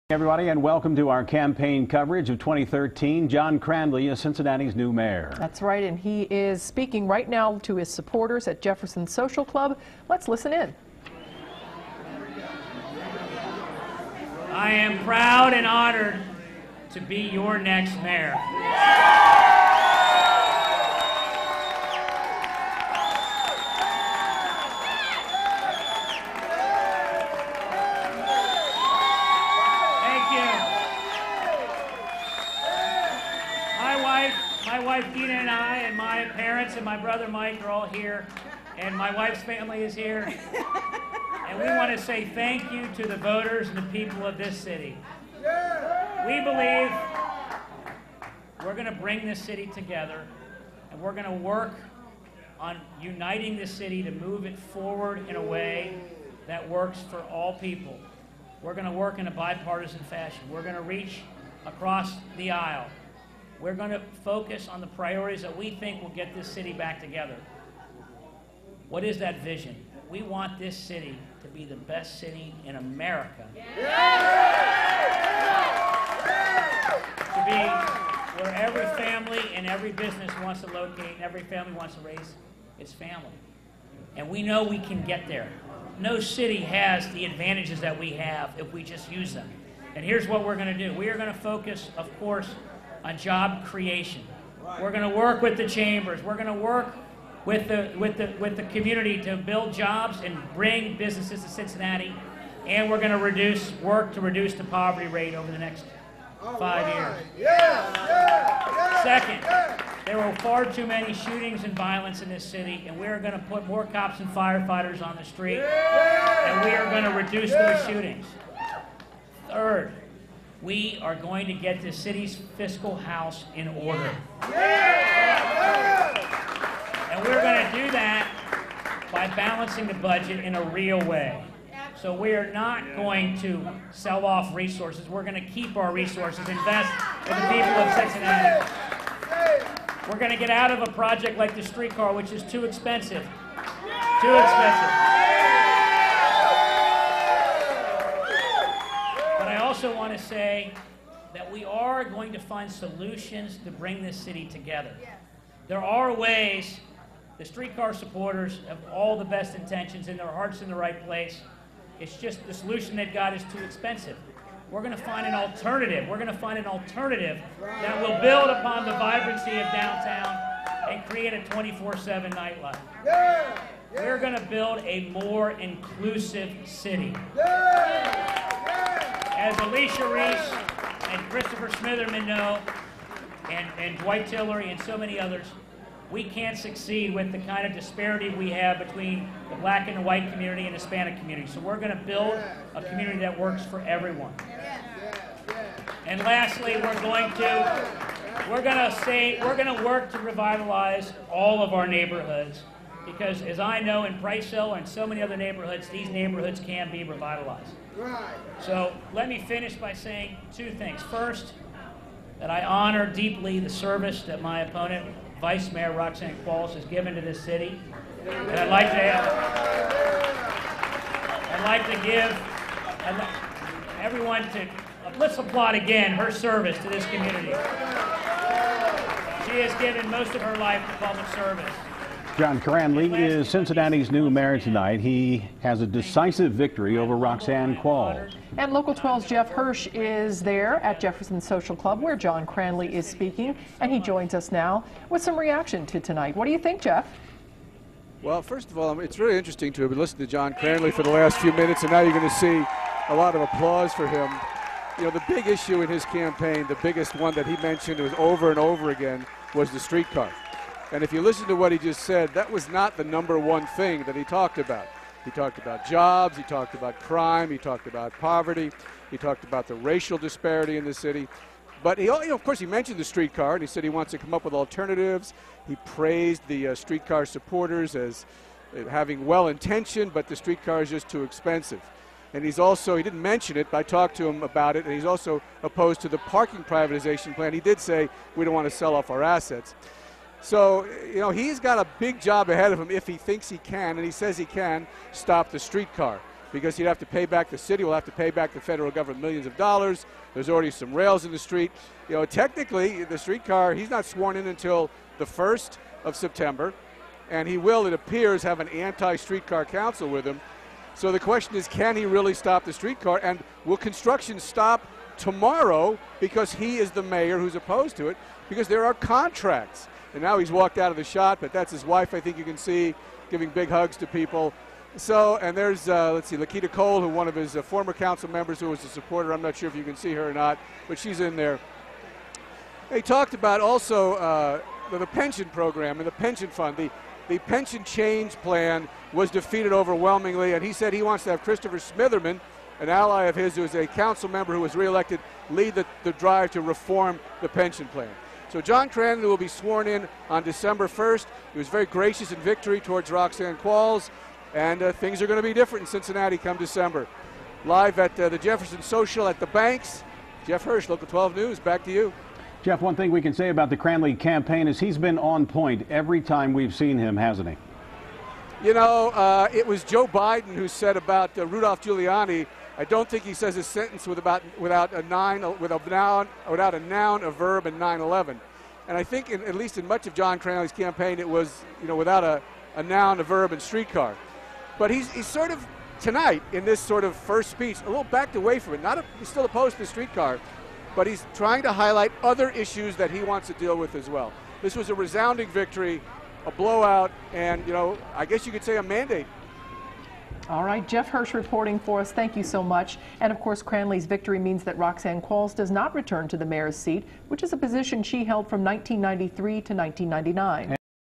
John Cranley Speaks After Being Elected Mayor of Cincinnati
CINCINNATI (Jeff Hirsh) -- John Cranley defeated Roxanne Qualls by a decisive margin. Cranley addressed his supporters in downtown Cincinnati during an acceptance speech at Jefferson Social.  Local 12's Jeff Hirsh reports.